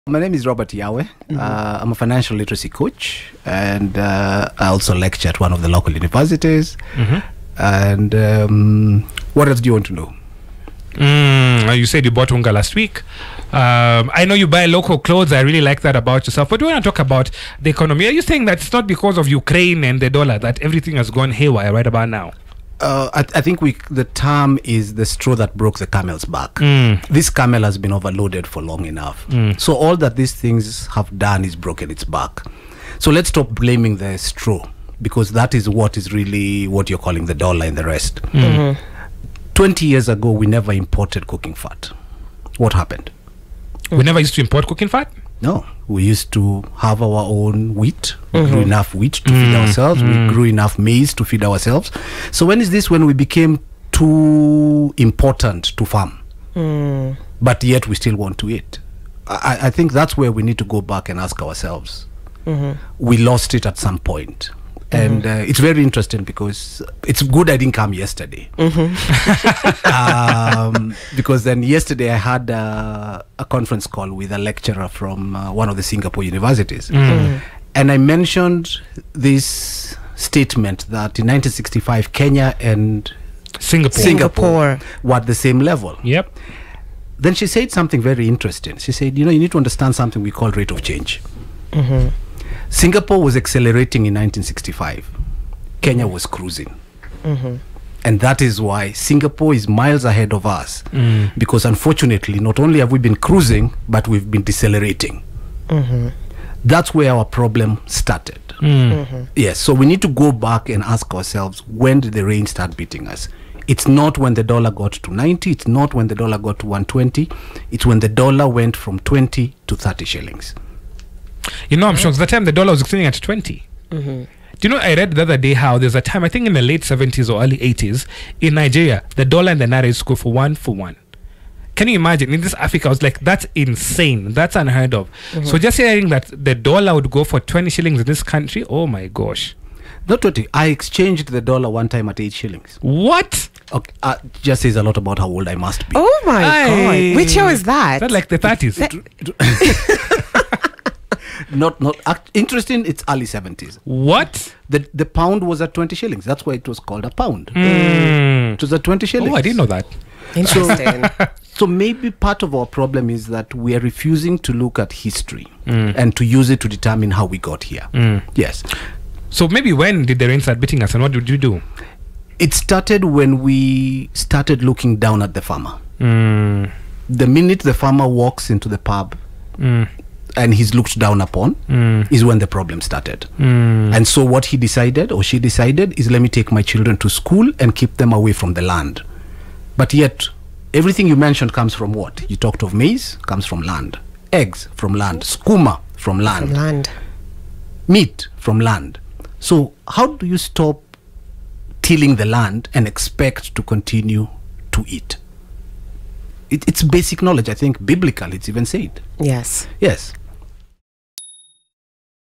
WHY ARE WE LOOKING DOWN AT A FARMER WHO CAN CHANGE KENYA'S ECONOMIC STATUS ? /Robert Yawe
WHY ARE WE LOOKING DOWN AT A FARMER WHO CAN CHANGE OUR ECONOMIC STATUS AS A COUNTRY? / Robert Yawe/